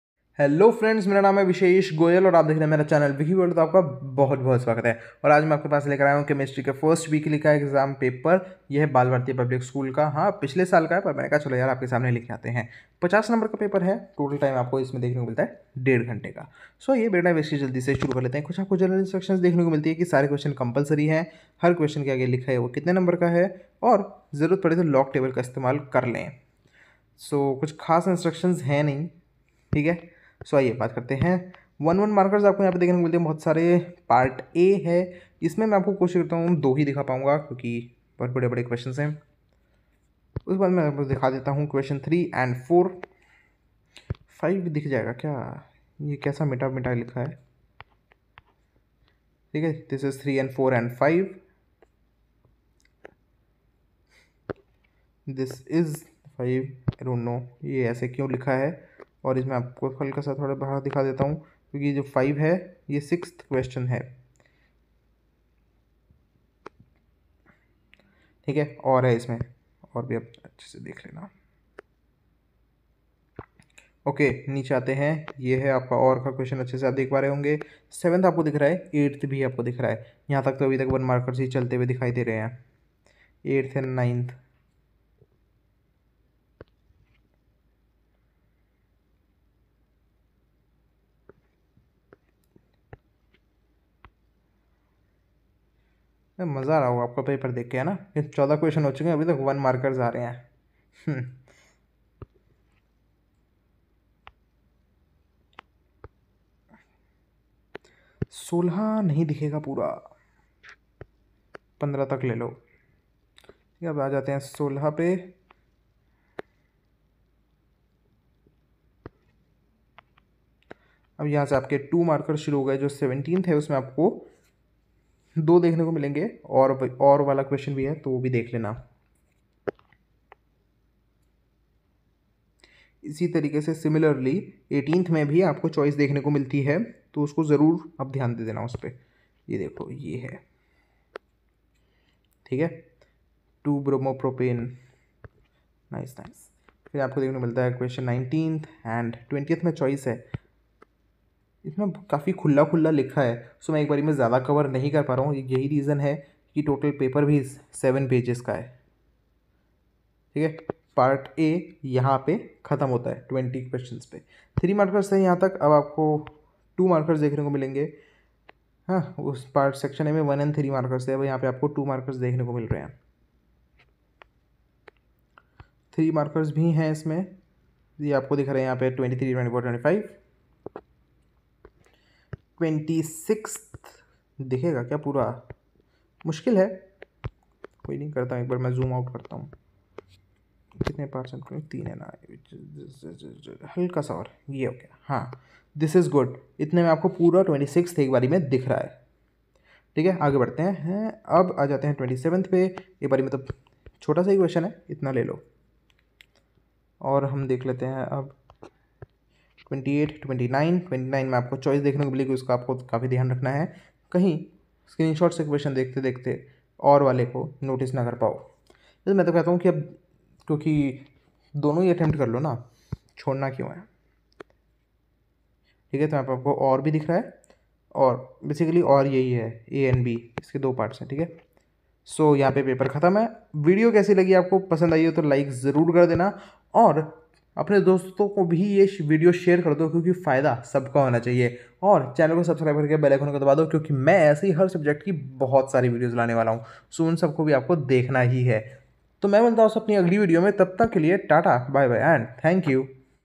Class 12 Chemistry - first weekly paper - Bal Bharati Public School
Full question paper of class 12 CHEMISTRY First Weekly examination of Bal Bharati Public School. (2020-21)

Follow Wiki World on Instagram!